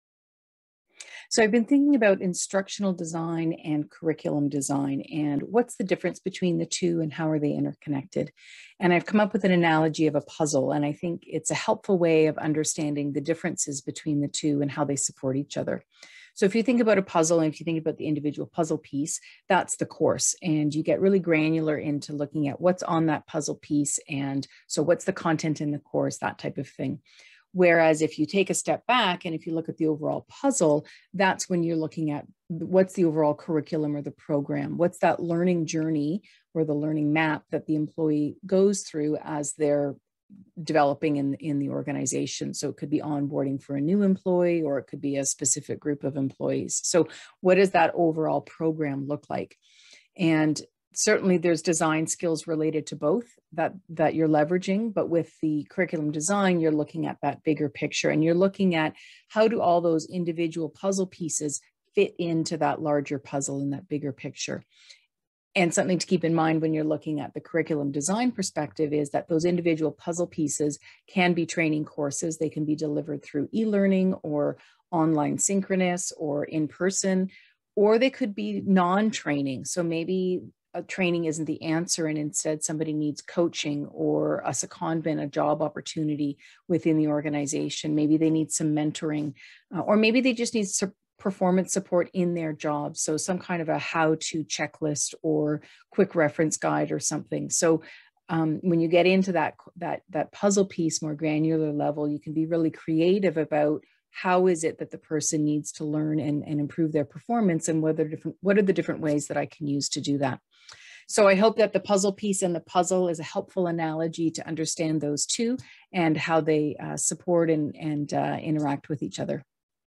Curriculum Design and Instructional Design
What's the difference between Instructional Design and Curriculum Design?
Here's a puzzle analogy to explain the difference between the two.
Start your New Year by meeting other L&D professionals and gaining fresh insights. Join me in my two upcoming webinars to get your creative juices flowing. Find out more in the comments.

► Learning Strategy - Jan 18 2:30 pm EST
► Design for Online - Jan 25 2:30 pm EST